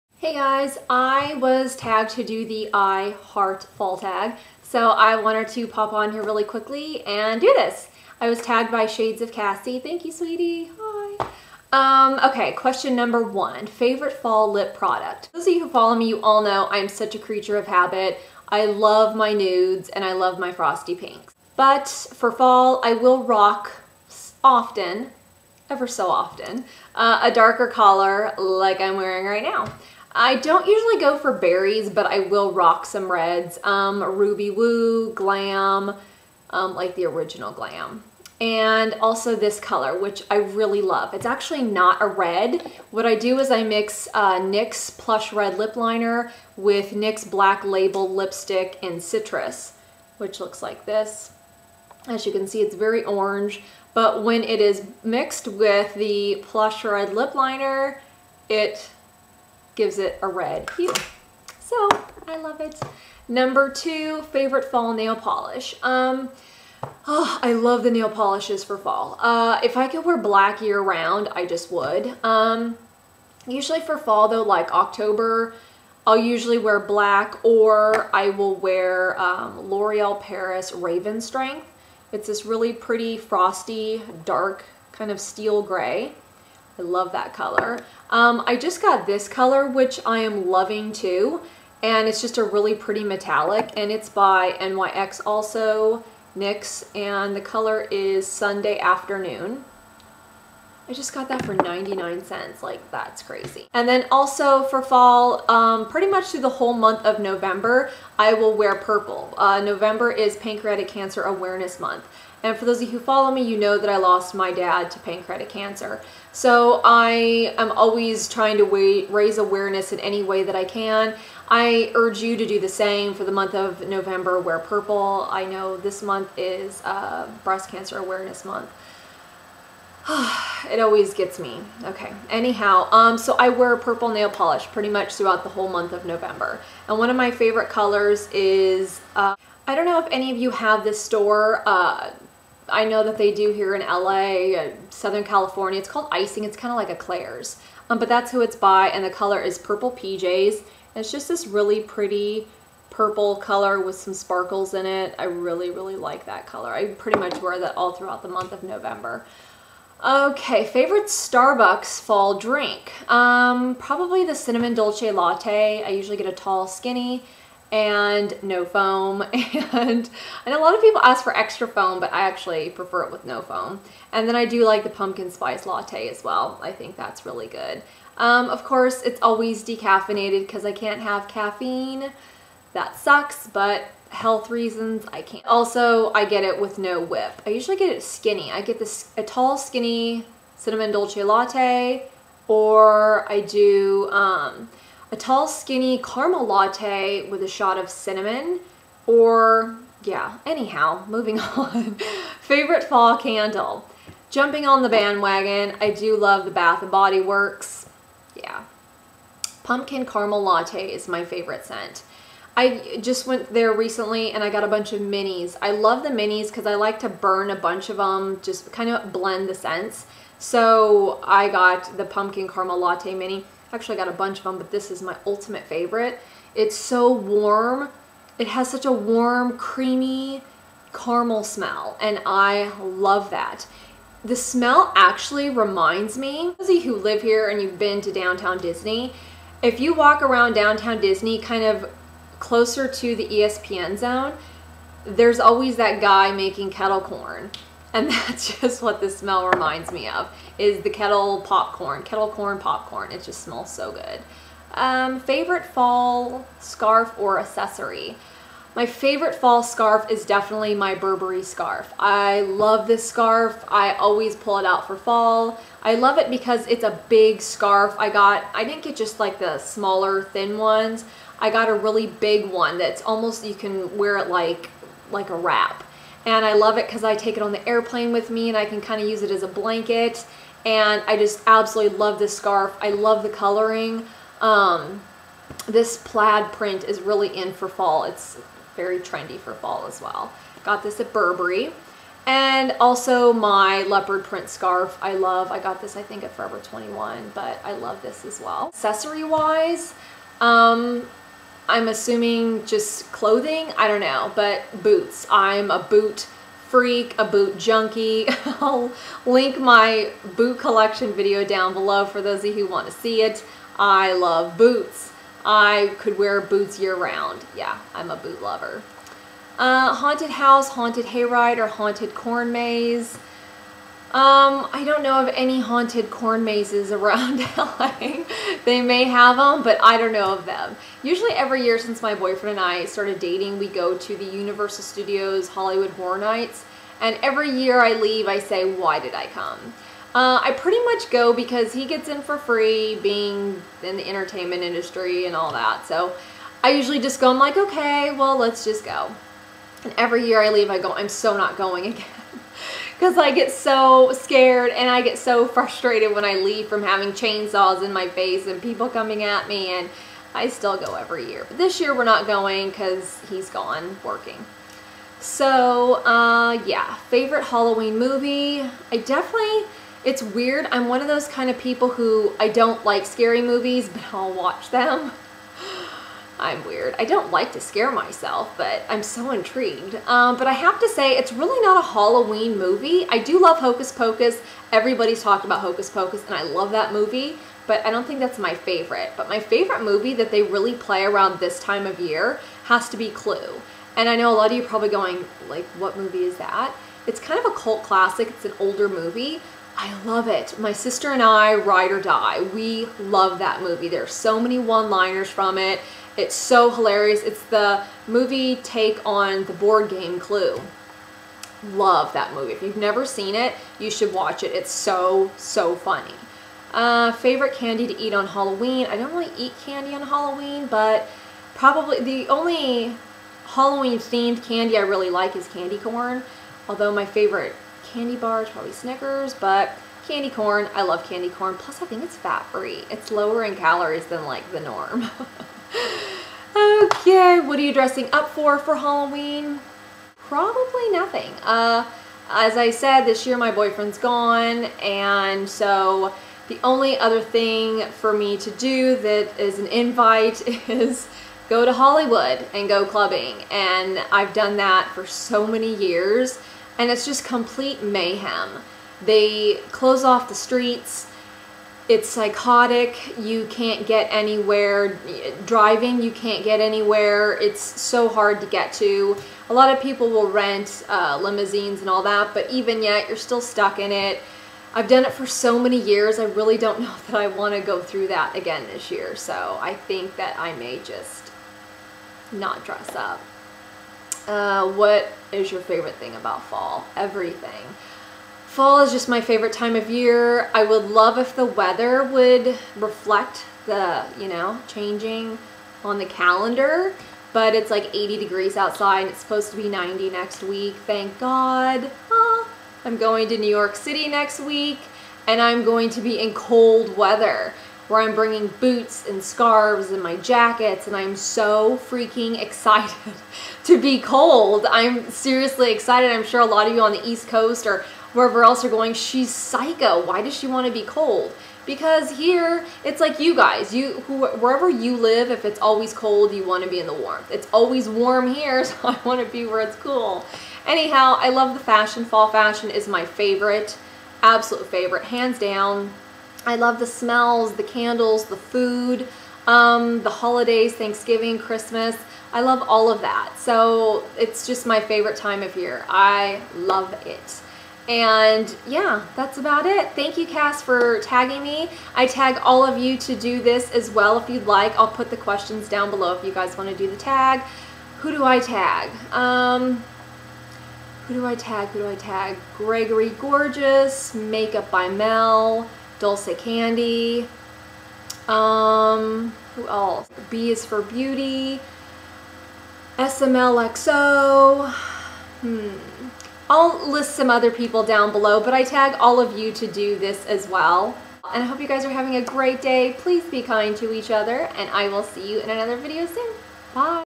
I ♥ Fall TAG!
**OPEN ME***

I tag you all to do this if you'd like!

QUESTIONS:
1 Favorite Fall lip product?
2 Favorite Fall Nail Polish?
3 Favorite Starbucks Fall Drink?
4 Favorite Fall Candle?
5 Favorite Fall Scarf or accessory
6 Haunted house, haunted hay ride, or haunted corn maze?
7 Favorite Halloween movie?
8 Favorite candy to eat on Halloween?
9 What are you dressing up as for Halloween?!
10 What is your Favorite thing about Fall?